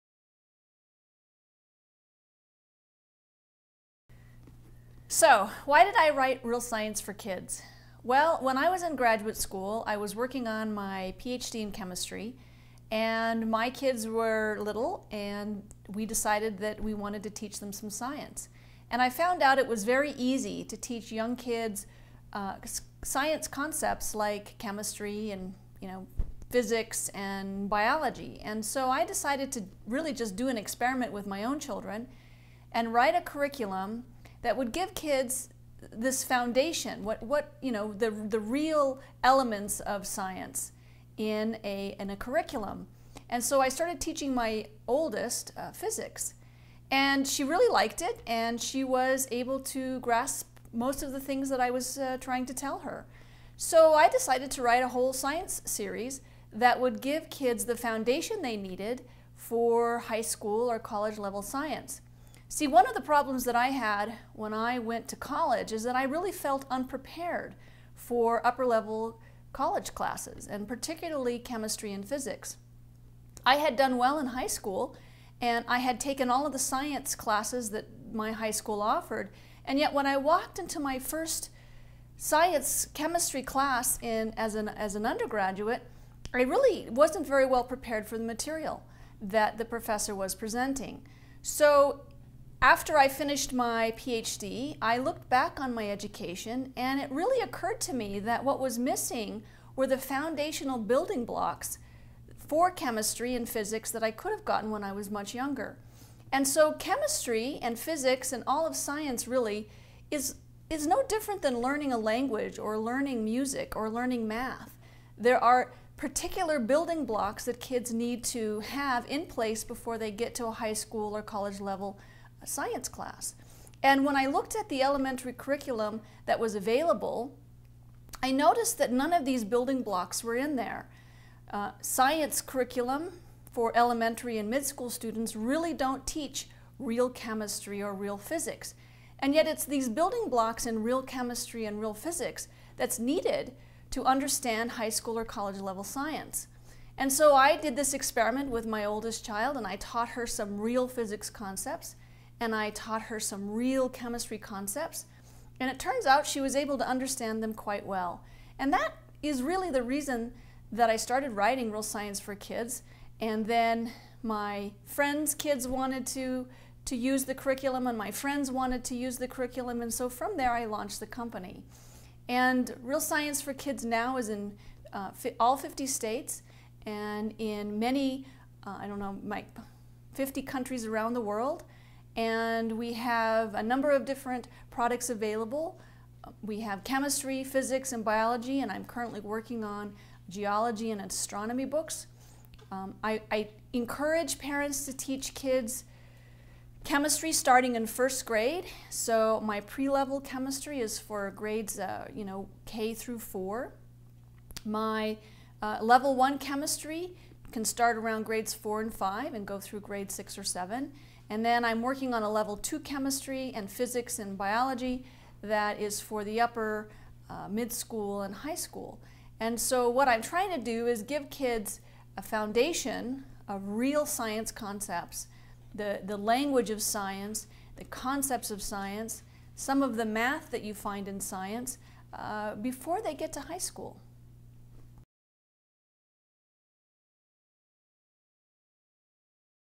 Real Science for Kids Creator Rebecca Keller
Dr. Rebecca Keller of Gravitas Publications discusses Rea l Science for Kids and what inspired her to start home school science curriculum's for kids, so they could study geology, biology, physics and all other forms of science in their home schooling classes